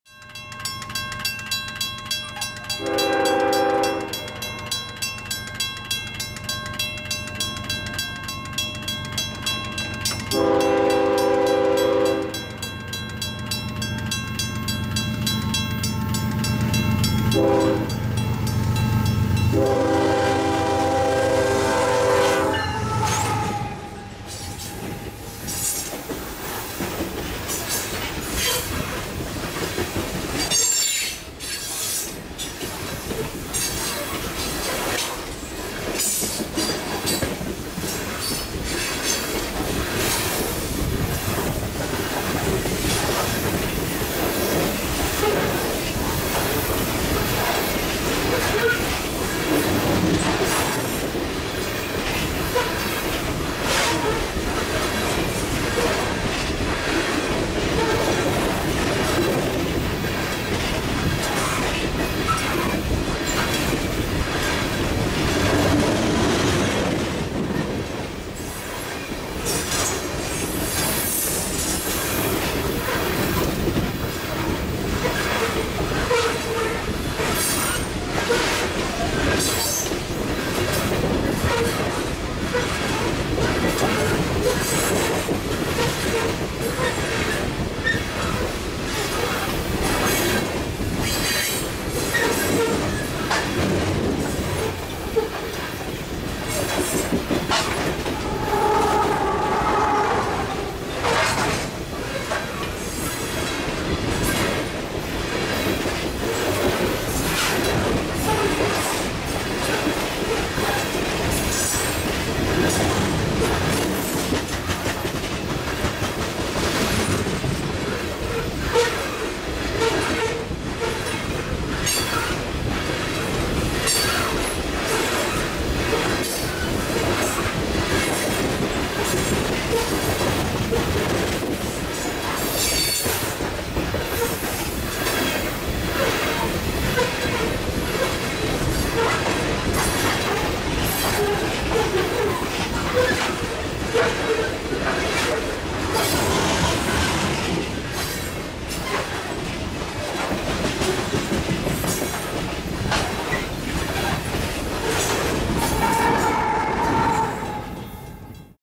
CP Westbound Coal at 184th Street in Surrey August 23rd 2009
Keywords: Train Trains Video Railroad Crossing Railfan Railfanning Railway Crossing Canada British Columbia BC Metro Vancouver Fraser Valley